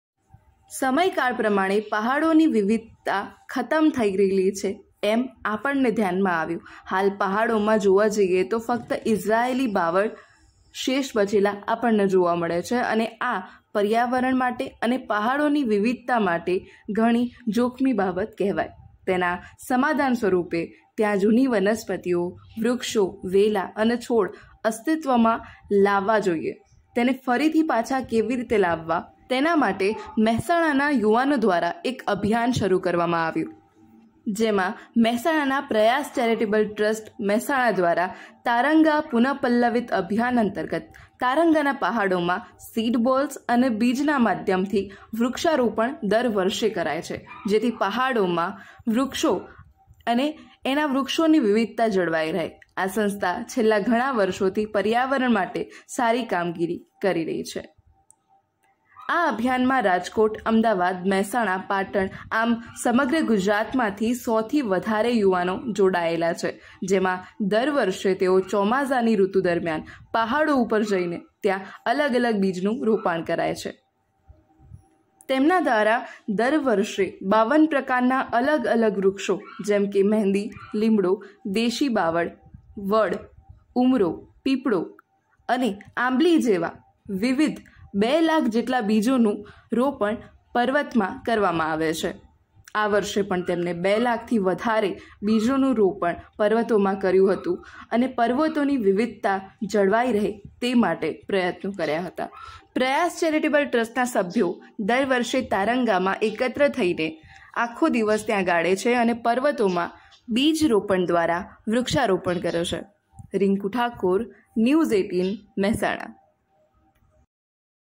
Taranga Hill | પર્વતોની વિવિધતા જાળવવા કરવામાં આવે છે અનોખું કાર્ય, જુઓ Video
Taranga Hill | A unique work is done to preserve the diversity of the mountains, see Video

સમય કાળ પ્રમાણે પહાડોની વિવિધતા ખતમ થઈ રહી છે. ત્યારે મહેસાણામાં કાર્યરત પ્રયાસ ચેરીટેબલ ટ્રસ્ટ દ્વારા પહાડોની વિવિધતા જળવાય તે માટે તારંગા પુનઃ પલ્લવિત અભિયાન અંતર્ગત તારંગાના પહાડોમાં સીડ્સ બોલ અને બીજના માધ્યમથી વૃક્ષારોપણ કરાય છે. આ સંસ્થામાં ગુજરાતમાંથી લગભગ 100 જેટલા કાર્યકર્તાઓ જોડાયેલા છે. મહેસાણા જિલ્લામાં આવેલું તારંગા હિલ ઐતિહાસિક ડુંગર છે. આ પહાડ પર પહેલા વિવિધ વનરાજી જોવા મળતી હતી. સમય અંતરે પર્વતોની જુની વનસ્પતિ, વૃક્ષો,વેલો,છોડો અસ્તિત્વમાં રહ્યા નથી. આ વિશેષ વનસ્પતિઓ નામ માત્ર બચી છે.